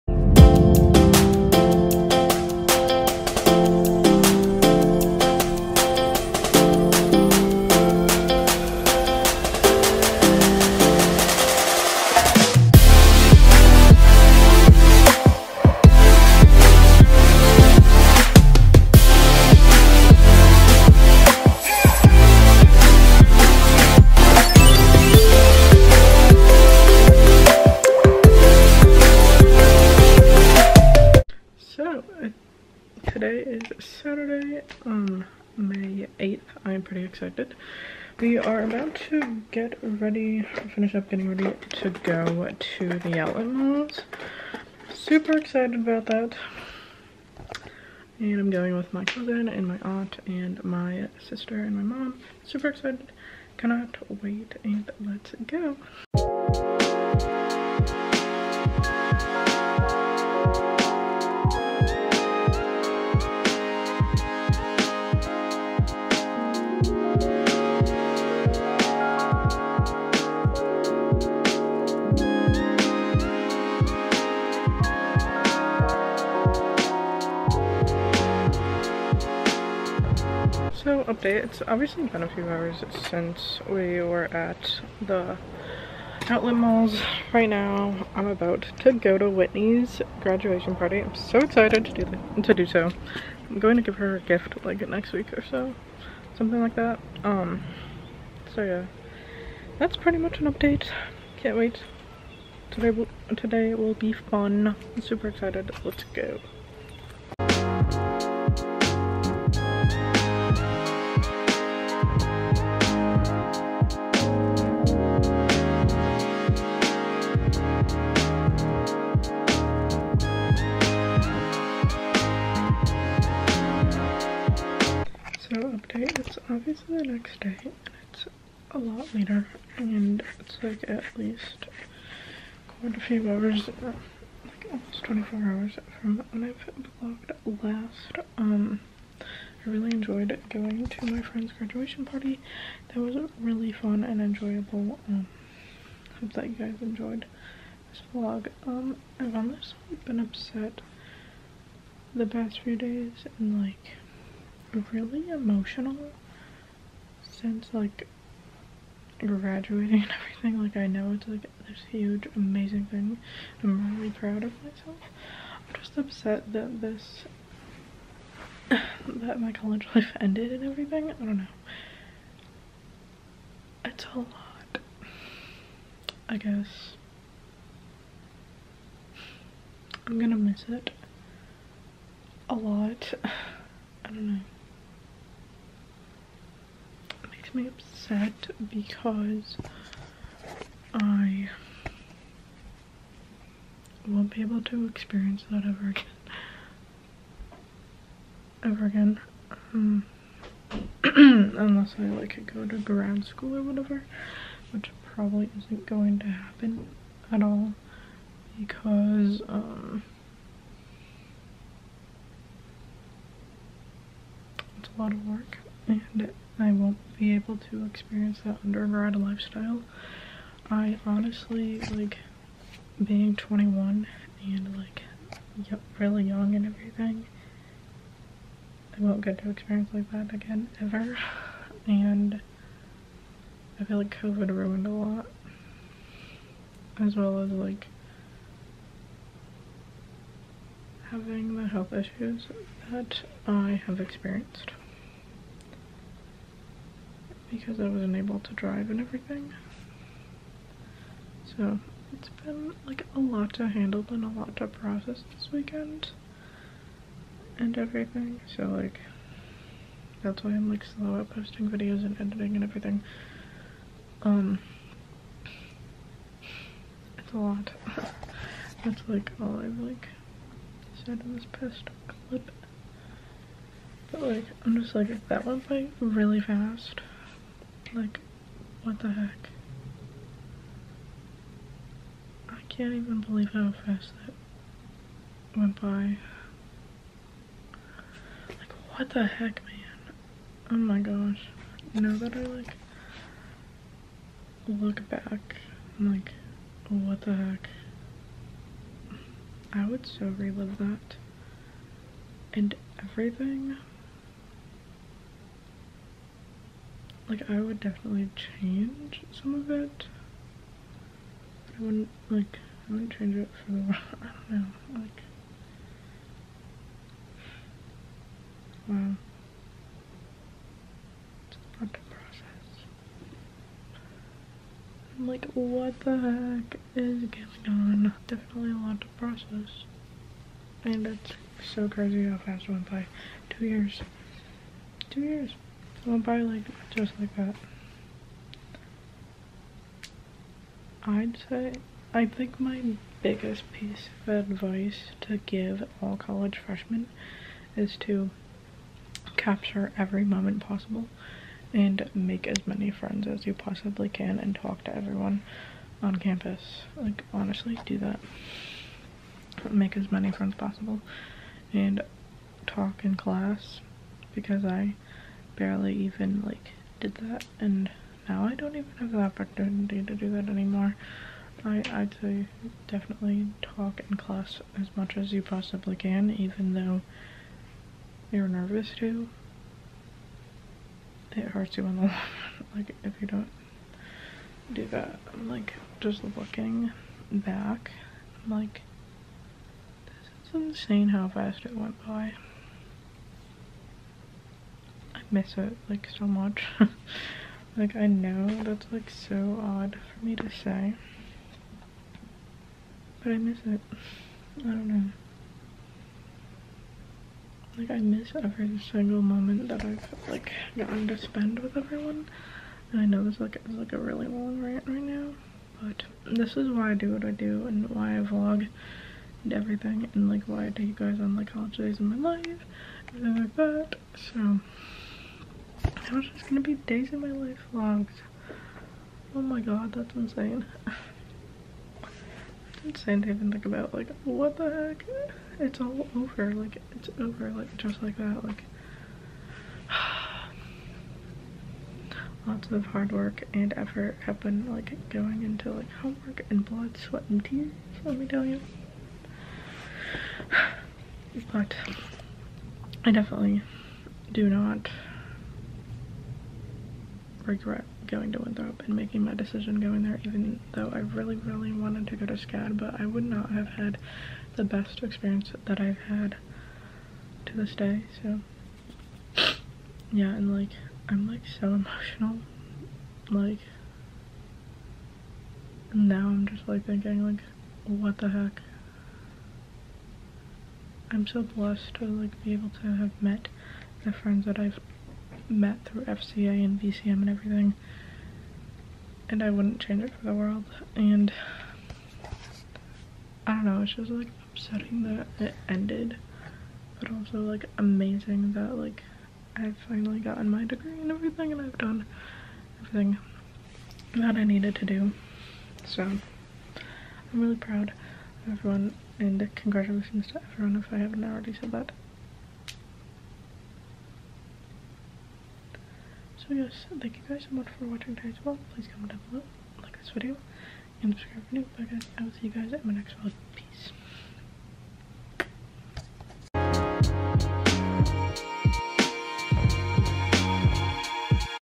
WEEKEND IN MY LIFE VLOG! Shopping, Graduation Party and Chat With Me | Hannah Rebekah
Hey my lovely supporters! This vlog is a WEEKEND IN MY LIFE VLOG! I went shopping with my family as well as go to a friend's graduation party that was short and sweet! I hope that you guys enjoy this vlog as much as I did editing it! Lmk what you all think in the comments below.